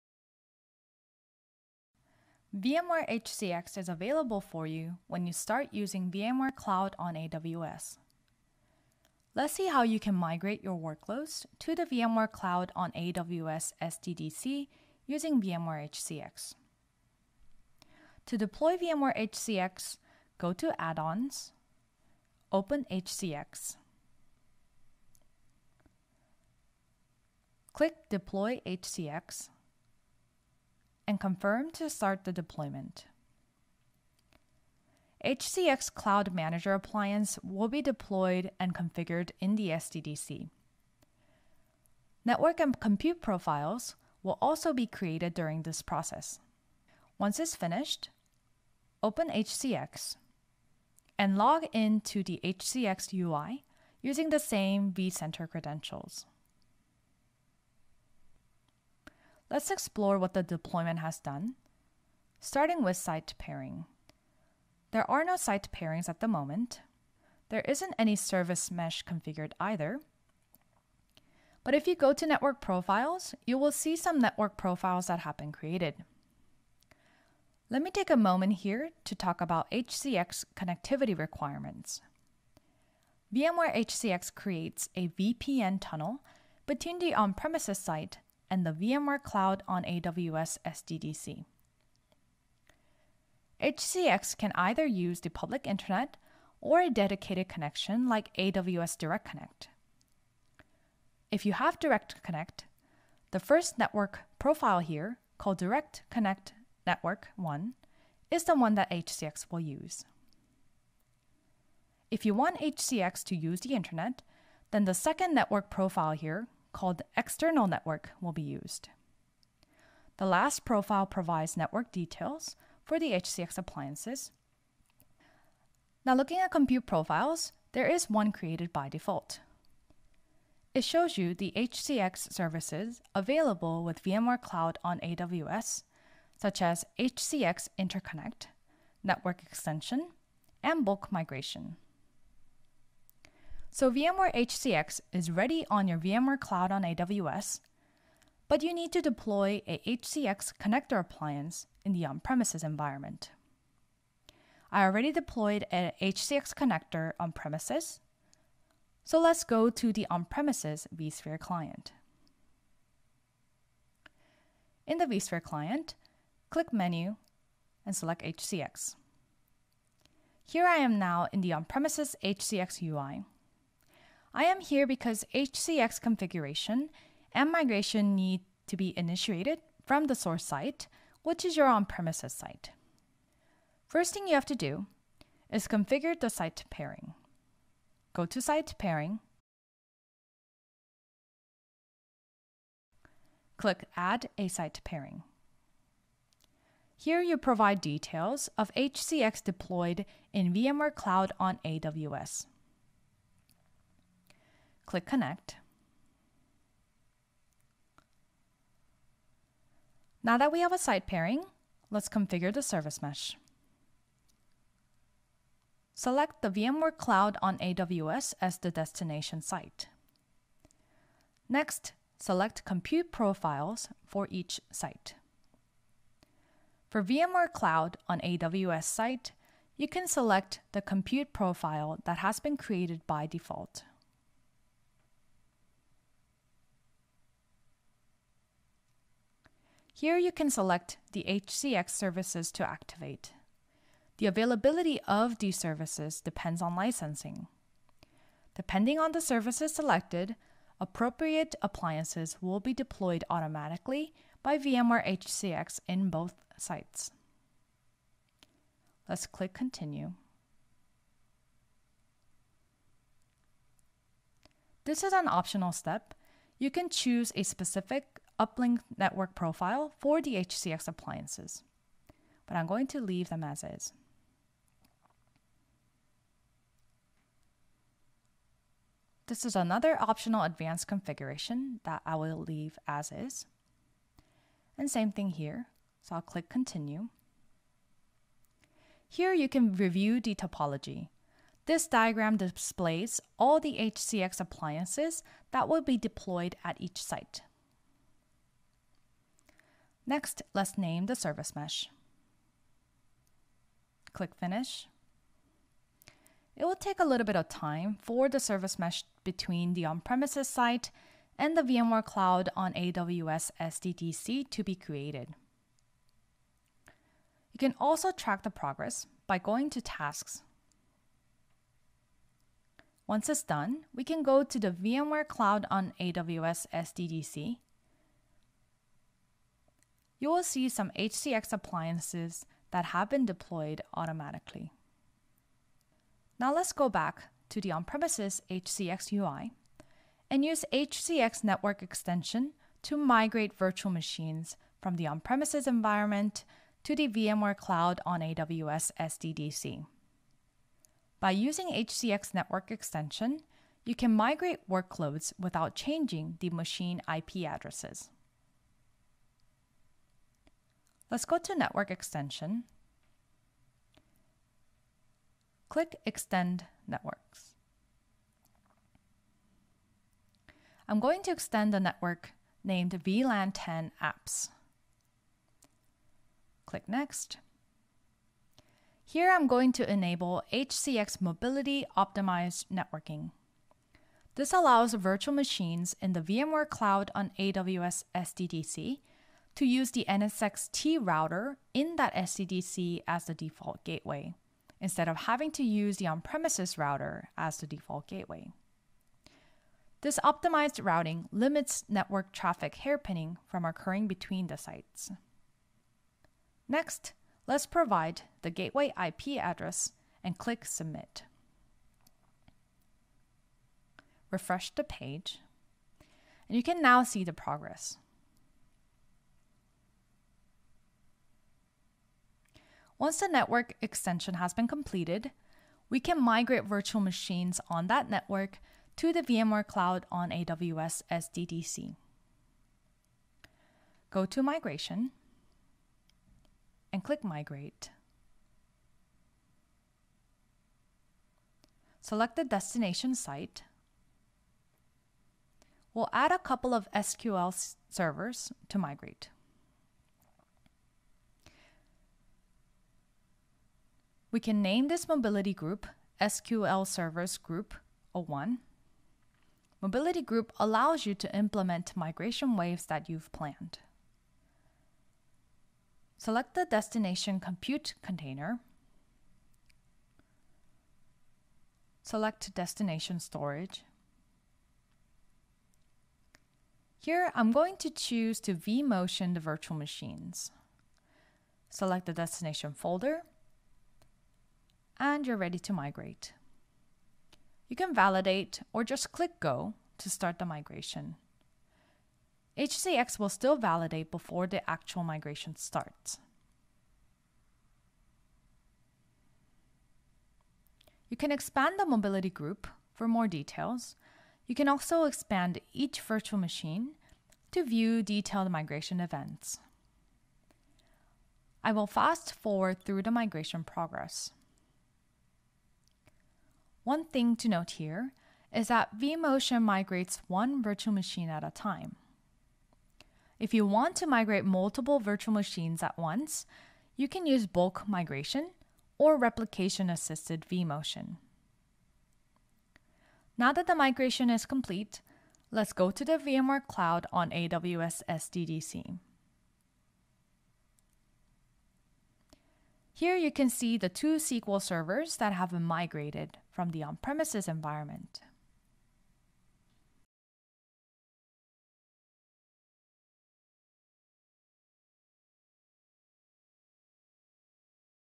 Migrating to a VMware Cloud on AWS SDDC Using VMware HCX
VMware HCX can seamlessly migrate vSphere workloads from an on-premises environment to a VMware Cloud on AWS SDDC. Here we highlight key capabilities of VMware HCX, such as Mobility Groups and Mobility Optimized Networking, and provide a demonstration of a vSphere workload migration leveraging Network Extension.